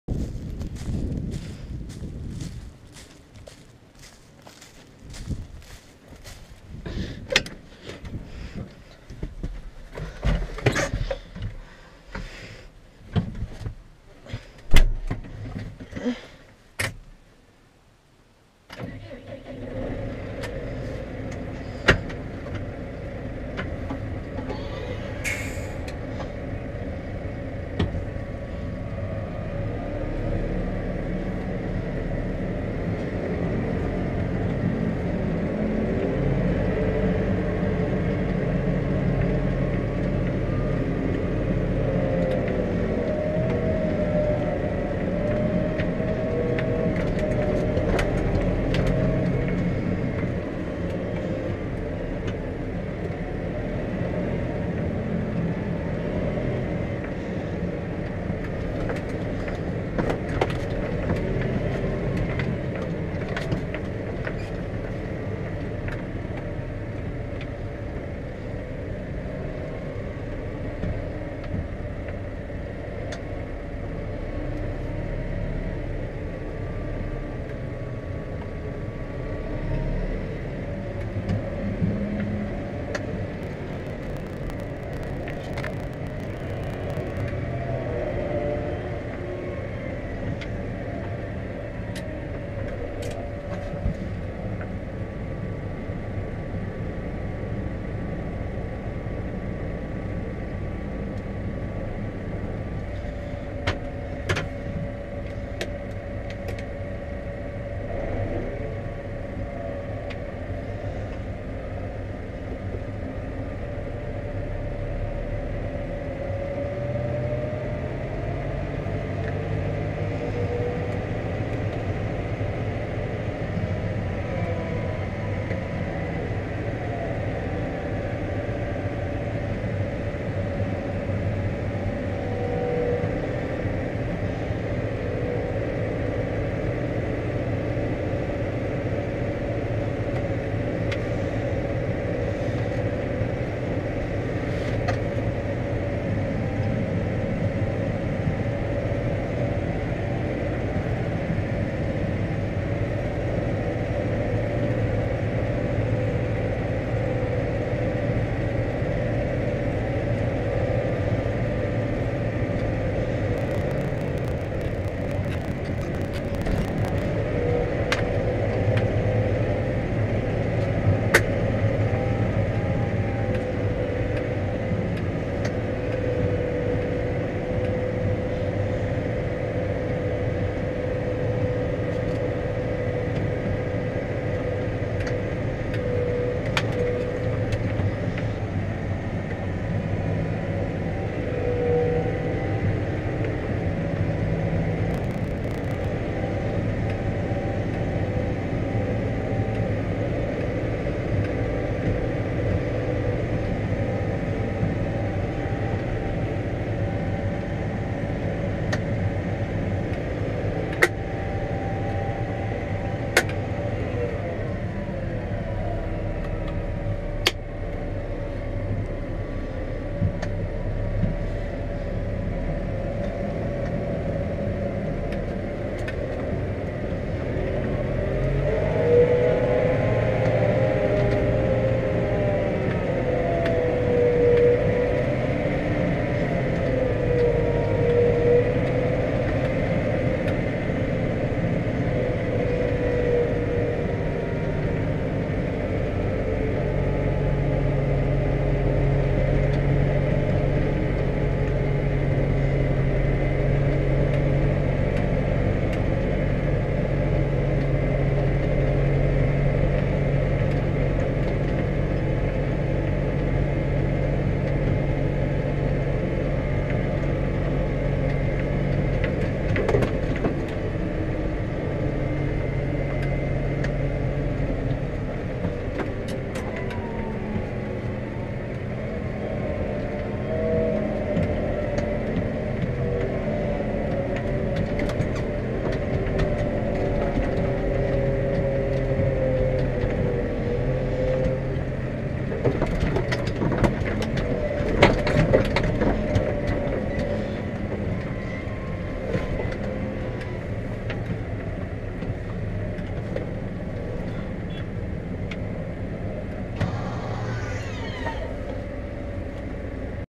POV Biogassubstrat Steuen|Steyr 4130 Profi CVT & Bergmann TSW 3140
In Diesem Video seht ihr wie wir im Lohn Biogassubstrat Gestreut Haben. Viel Spaß beim Anschauen